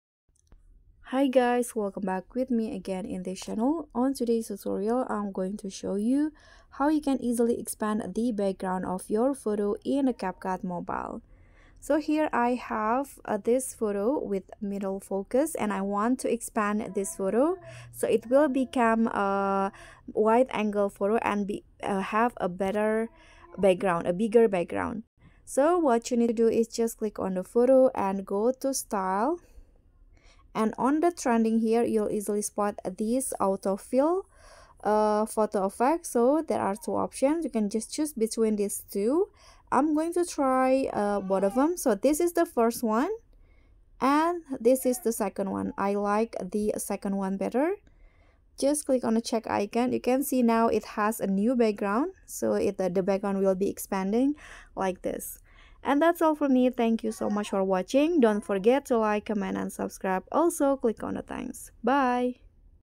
CapCut Photo Expansion Edit! How to Easily Expand the Background of a Photo in the CapCut Mobile?
Here's how you can do the photo expansion effect in CapCut Mobile